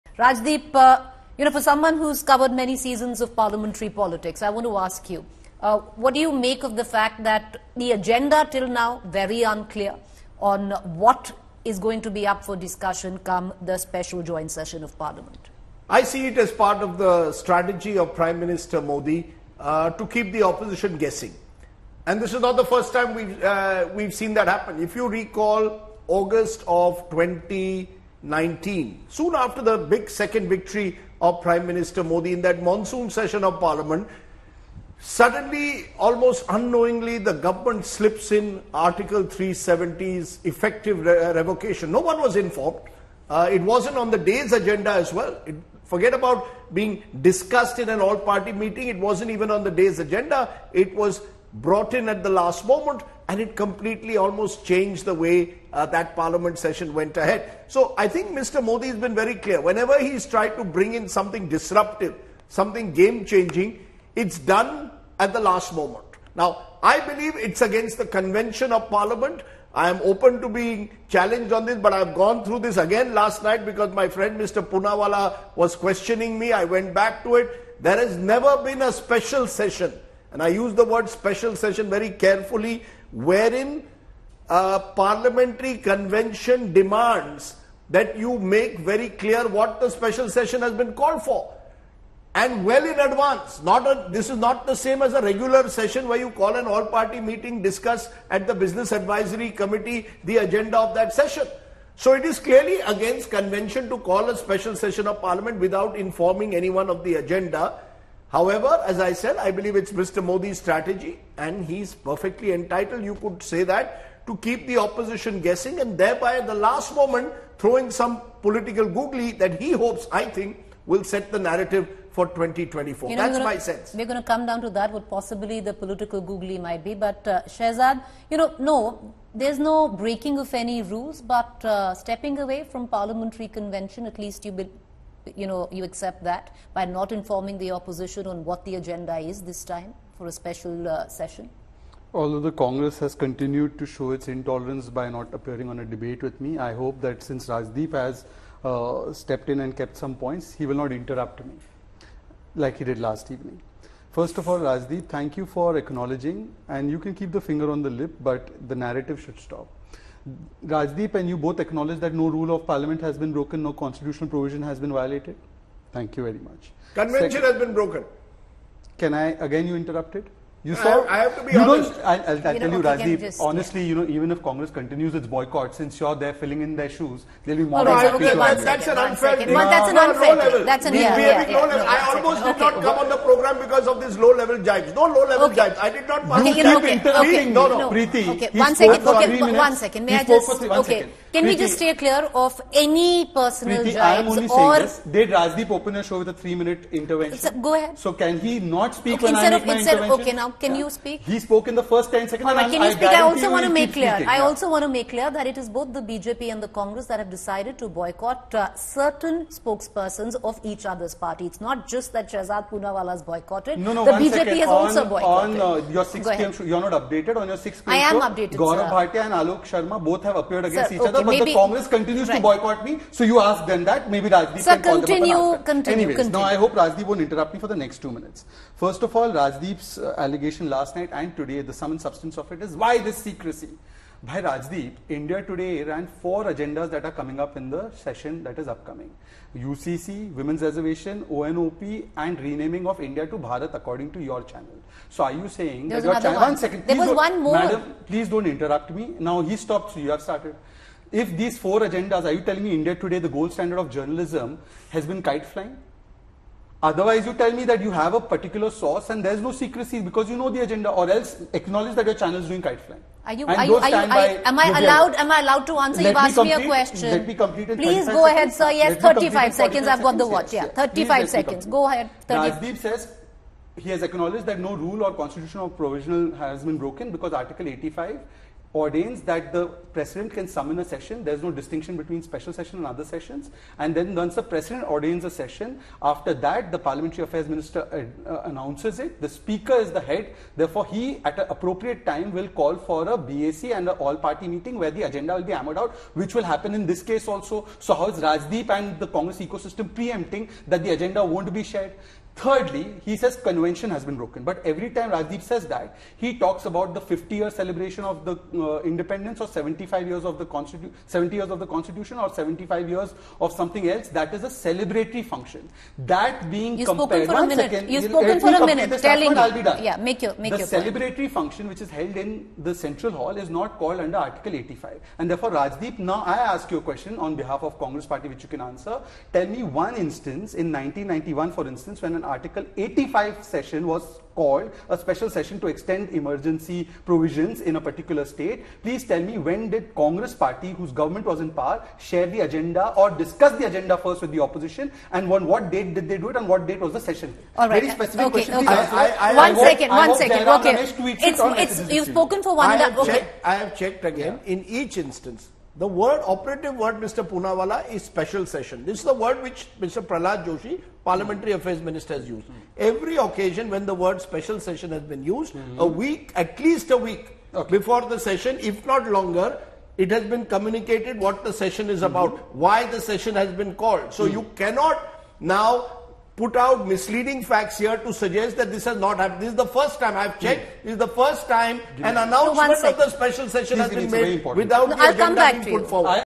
Rajdeep Sardesai & BJP's Shehzad Poonawalla Get Into A Heated Debate Regarding Special Parl Session
Parliament's special session, set to commence on September 18, will begin in the old building. On September 19, on the occasion of Ganesh Chaturthi, the parliamentary operations will shift to the new Parliament building, which was inaugurated by Prime Minister Narendra Modi on May 28 this year.